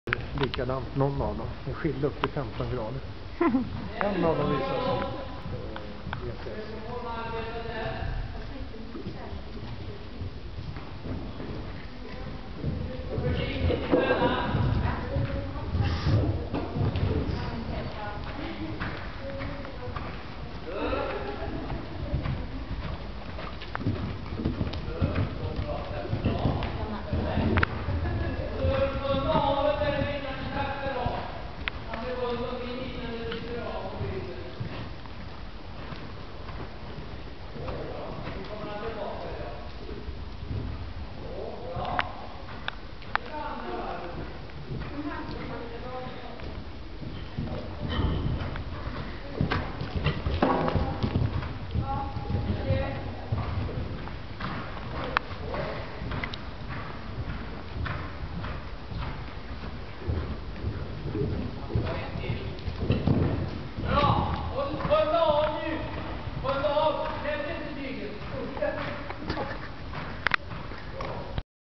Francis Rivis, Bosse-träning, 2012-02-22, del II
Francis Rivis hoppar bana på ca 110-120 cm.